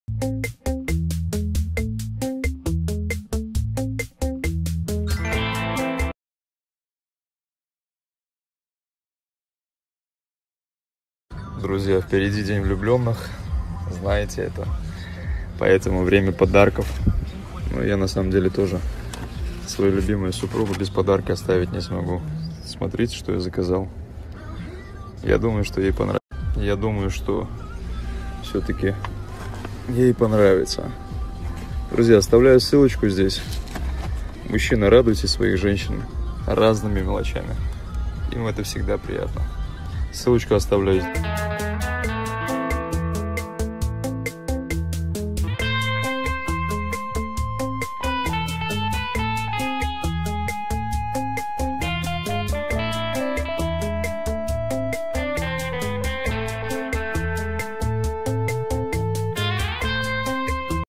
ПОДАРОК ГУСЕВА ДЛЯ РОМАНЕЦ НА 14 ФЕВРАЛЯ (ondom2.com)
В этом выпуске новости и слухи  про Антона Гусева, который купил подарок Вике Романец на 14 февраля.